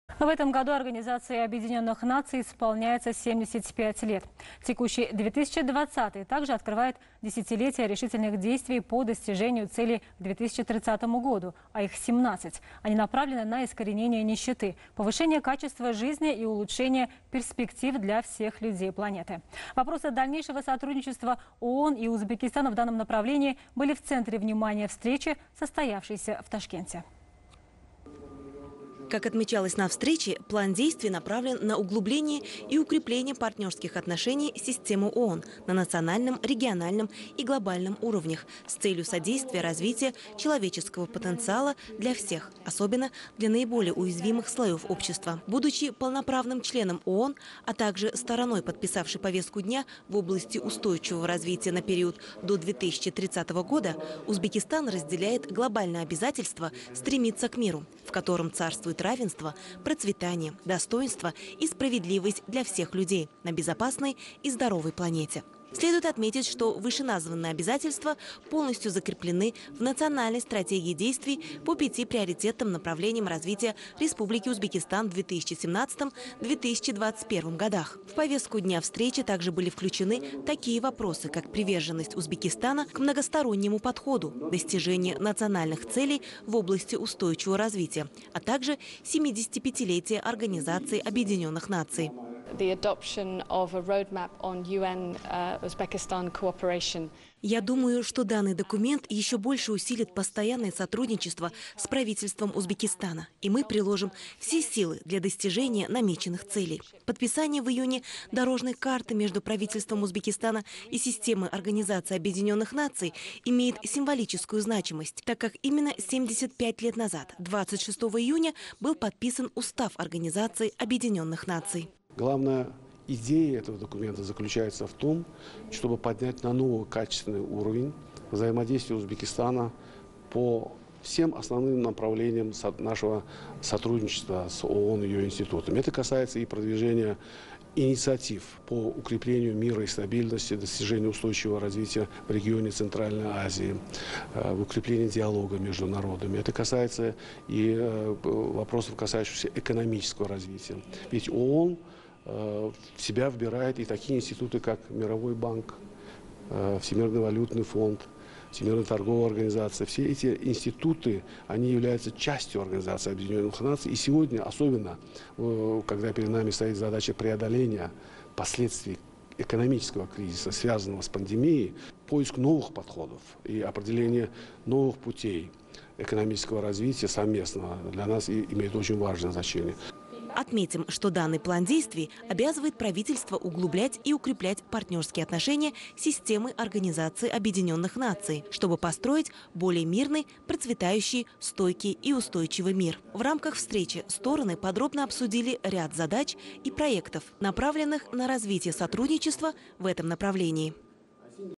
Обсуждение проекта ООН в Сенате Олий Мажлиса
Встреча постоянного представителя ООН в Узбекистане Хелены Фрейзер с первым заместителем Председателя Сената Олий Мажлиса Садыком Сафаевым